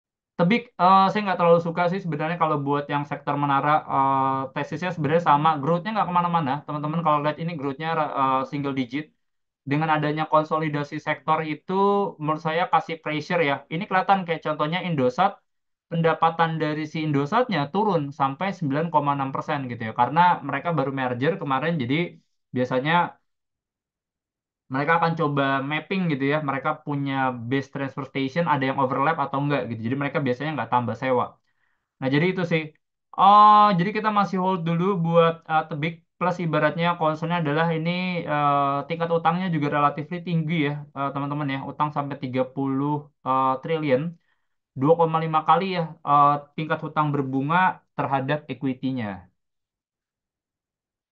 Review Laporan Keuangan Q2 2024 TBIG
Video ini merupakan cuplikan dari webinar rutin Komunitas HungryStock yang diadakan setiap Sabtu pagi.

Pada acara tanggal 10 Agustus 2024, HungryStock berkesempatan untuk bertemu dengan Pak Nico Laurens selaku Honorary Researcher HungryStock & Head of Research dari Panin Sekuritas pada acara StockScope & Review Laporan Keuangan.

Untuk bergabung bersama komunitas HungryStock bisa melihat info lebih lanjut di